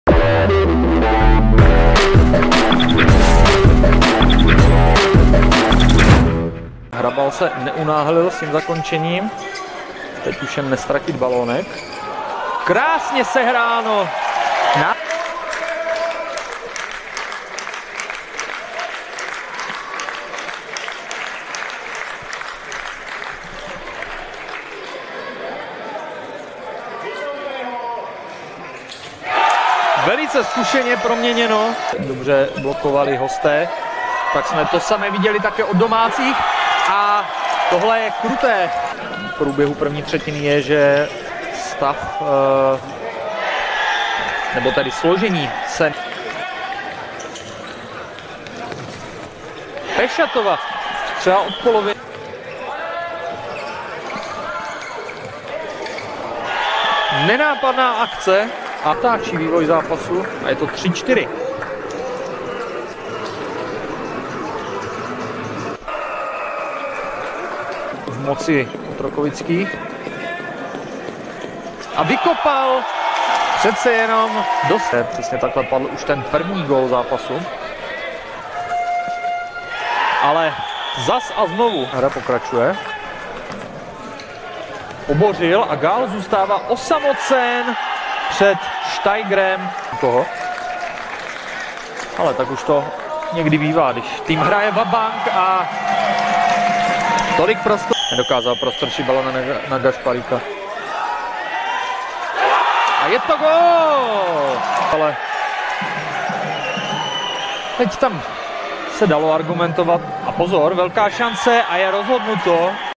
2. kolo | PSG PANTHERS OTROKOVICE - FBC ČPP Bystroň Group Ostrava 6:7p
Branky: 12. Matěj Kostka, 16. Ronald Gašparík, 20. Peter Nehila, 44. František Vykopal, 52. Ronald Gašparík, 57. Samuel Kelemen - 22. Marek Bräuer, 24. Lukáš Pešat, 29. David Friedel, 43. Lukáš Pešat, 47. Marek Bräuer, 50. Ladislav Gál, 64. Šimon Kuczera; rozhodčí: Lukáš Čížek - Lukáš Laštovička; vyloučení: 1:3; využití: 1:0; diváků: 221; zásahy brankářů: Miroslav Steiger ? - Jan Binder -6; po třetinách: 3:0, 0:3, 3:3, 0:1.